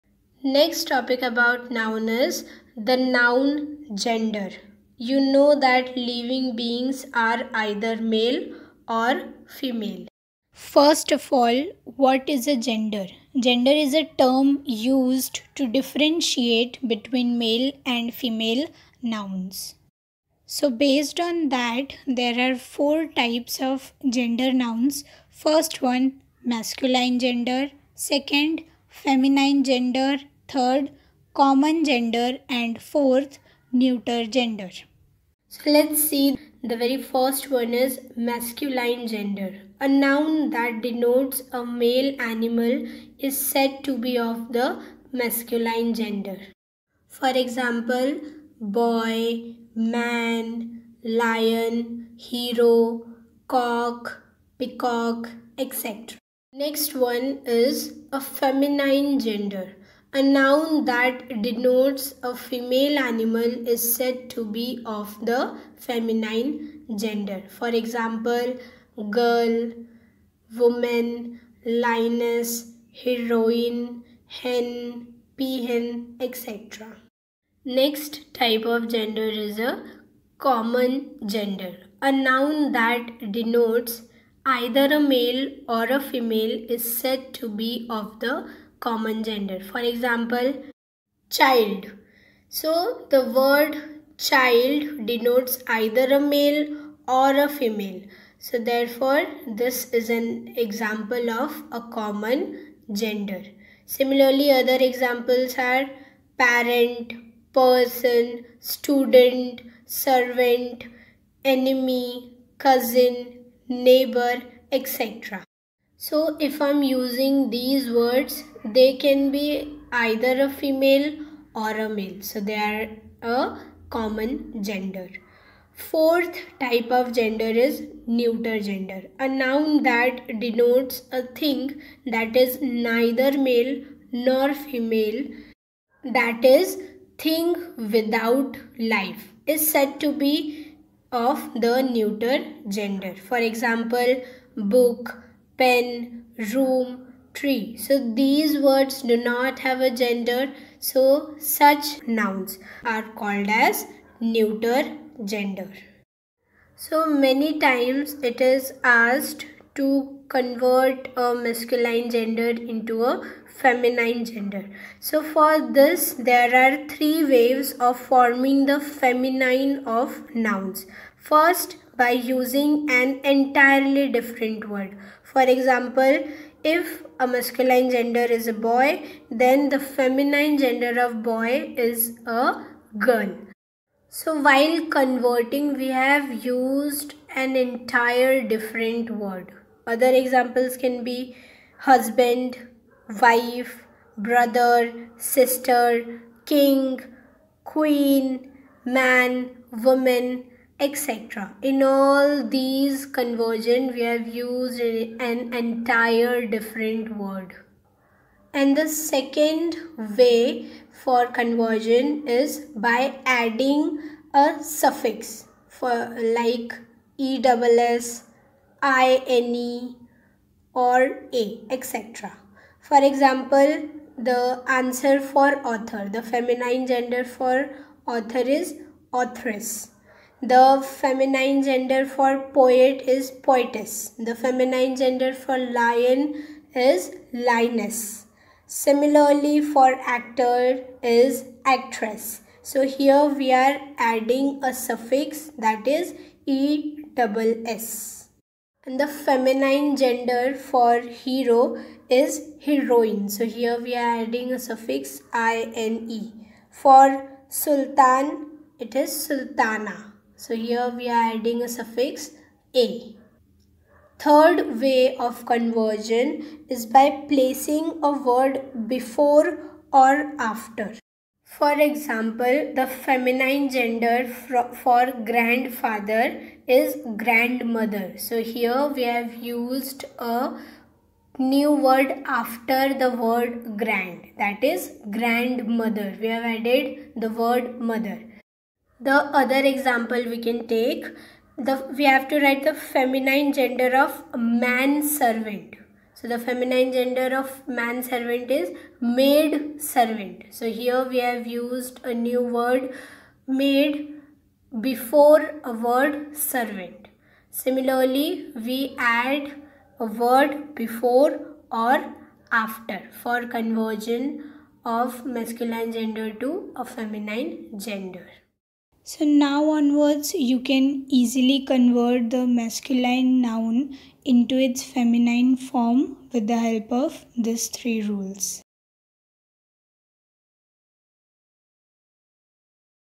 The Noun : Gender || Masculine, Feminine, Common and Neuter Gender || English Grammar
Hello everyone,
In this video I have explained 4types of Gender Nouns in English with examples, They are-
1) Masculine Gender
2) Feminine Gender
3) Common Gender
4) Neuter Gender